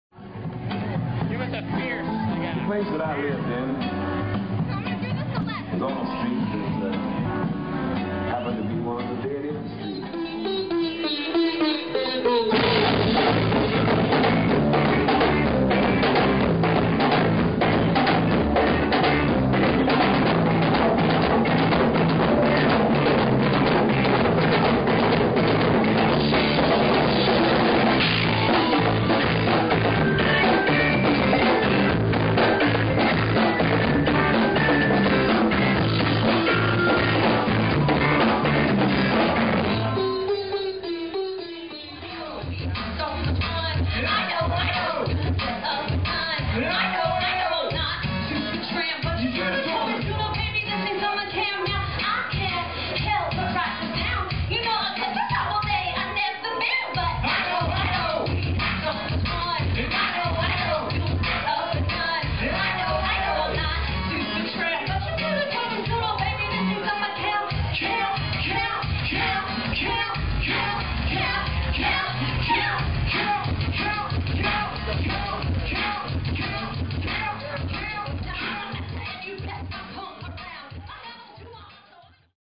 A-List: Insect Records party at Mohawk
Scenes from the Insect Records party at Mohawk on July 18, 2009. David Weaver/ FOR AMERICAN-STATESMAN.